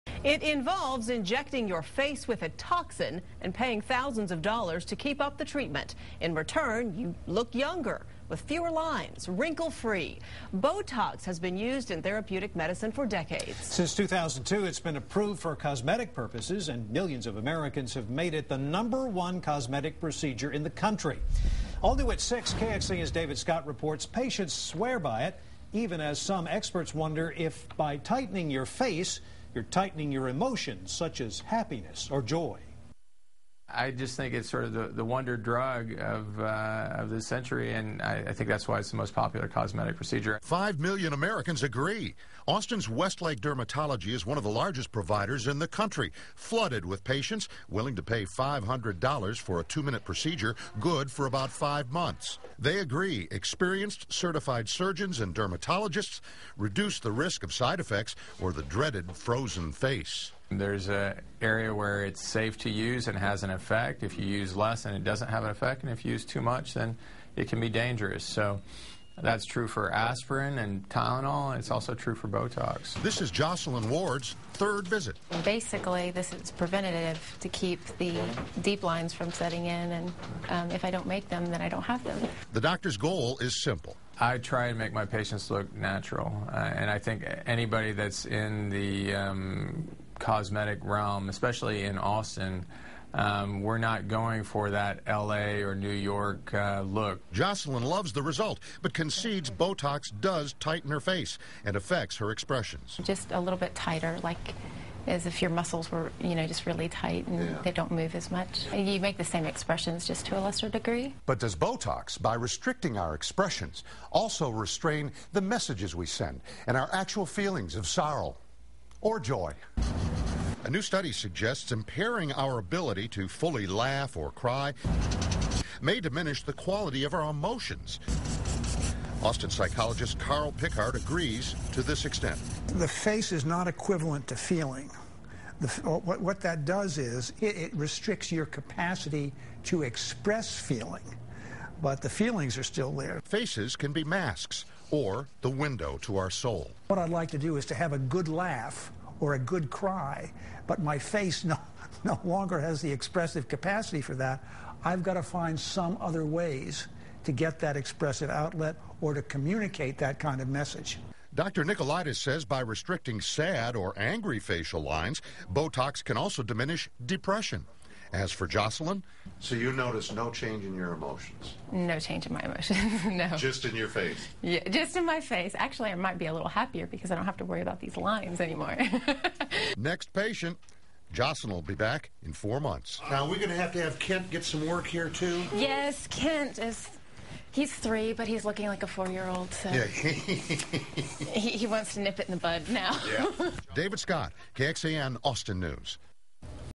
Botox emotional effects
Can the use of Botox have an impact on emotions?